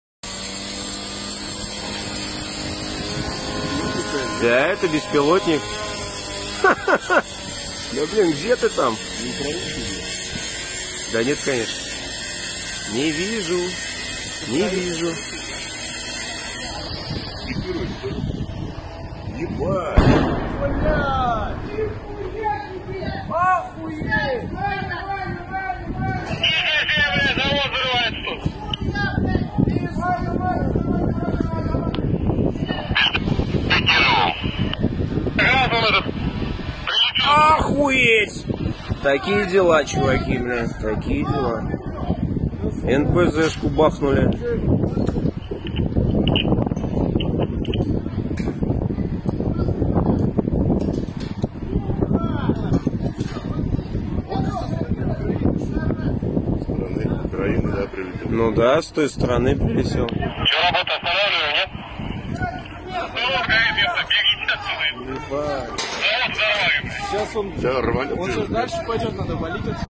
Ukrainian Drone Kamikaze Mugin-5 Pro Hits Russian oil refinery
Mugin-5 Pro 5000mm Carbon Fiber UAV Platform can hold up to 25Kg payload on board